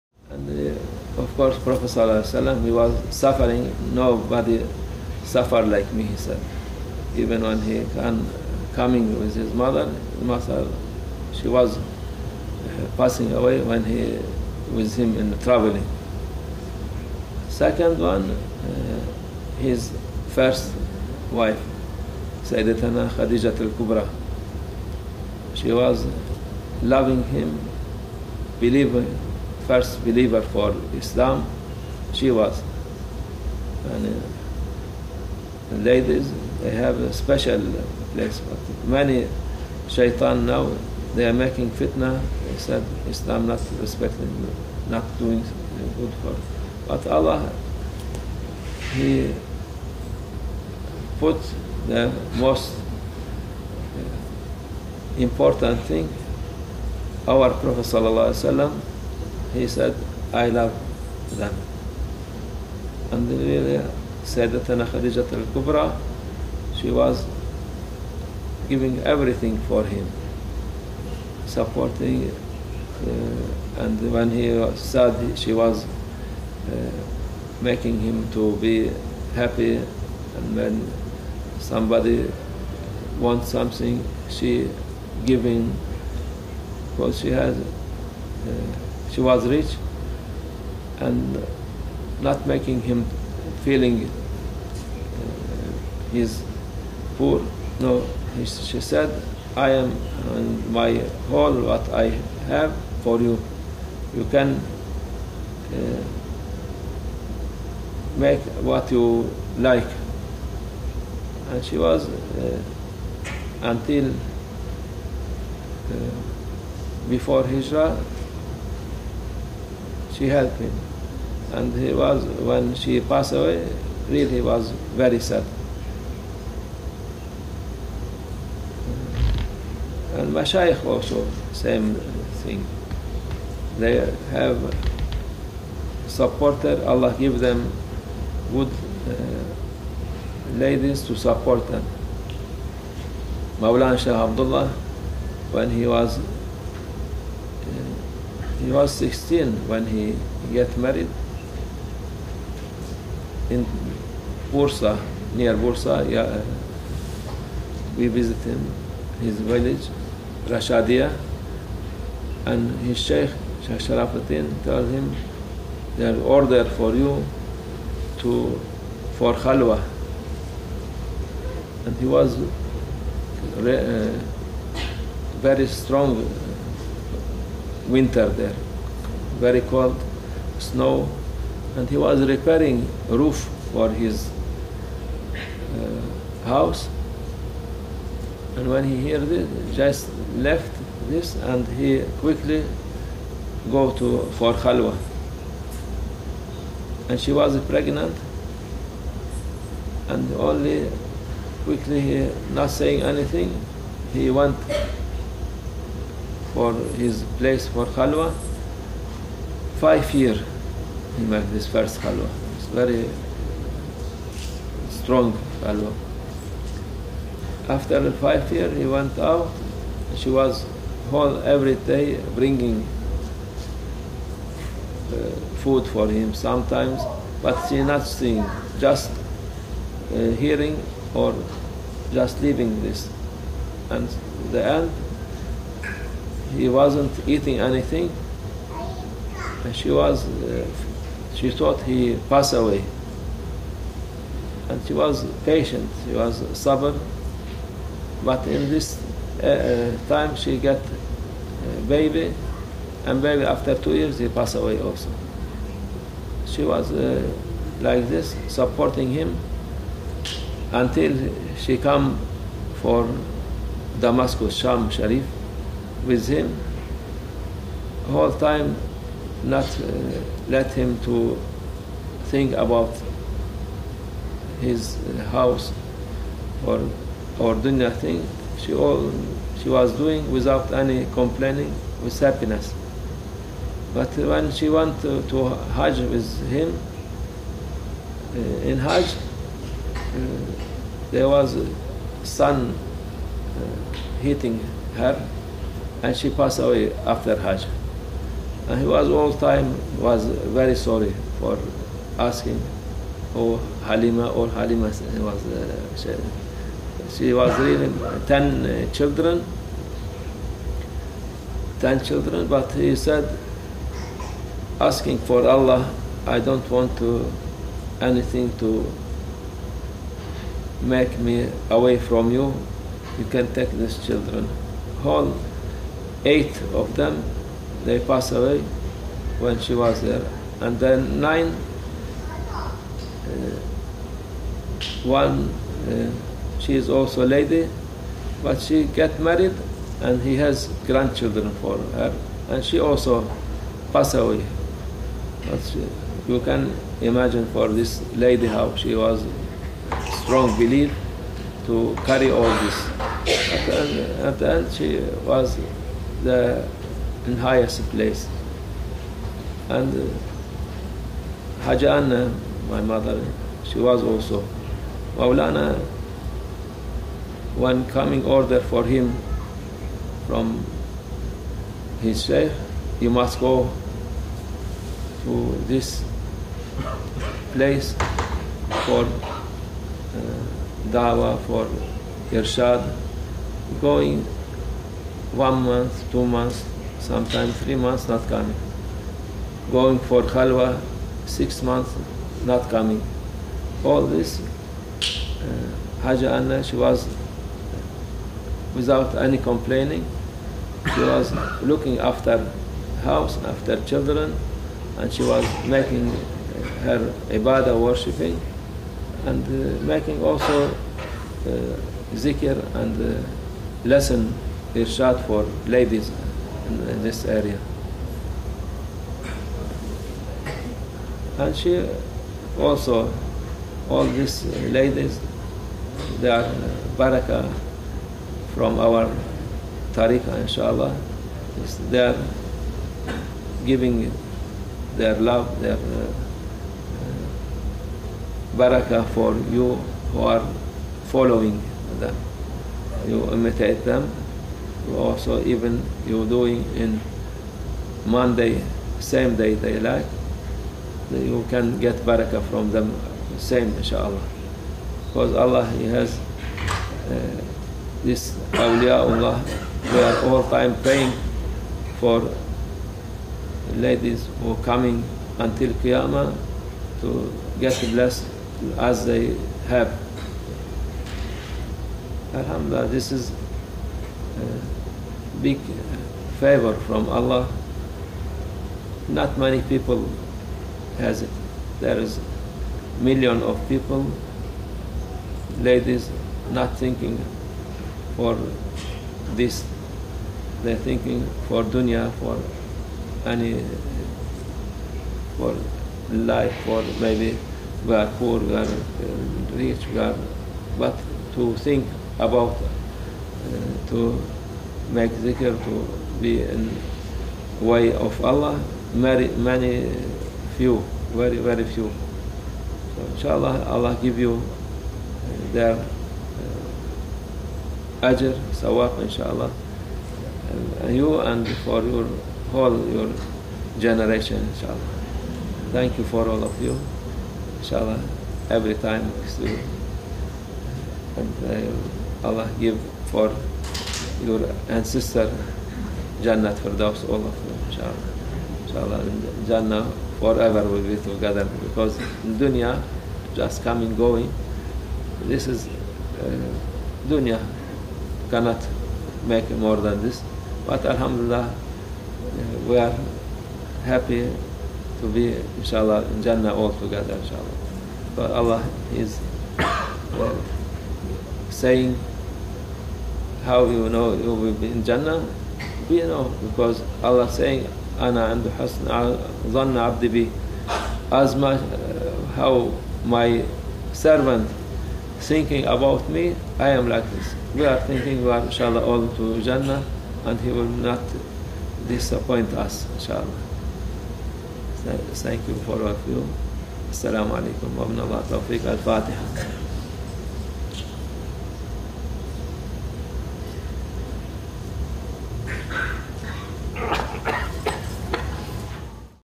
Шейх Мехмет Эфенди. Женщины Аулия
Присутствуют субтитры на русском языке.
Шейх рассказывает о женщинах святых. О Хатижетюль Кубре, о жене Шейх Абду-Ллах Халиме и жене Шейха Назима Хаджи Анне. Поддерживая своих мужей и выполняя обязанности в дунии, ибад и обучая других, они зарабатывали высокие стоянки и Рай. Мы можем получить их благословение, следуя их примерам.
Сухбат от 30.10.17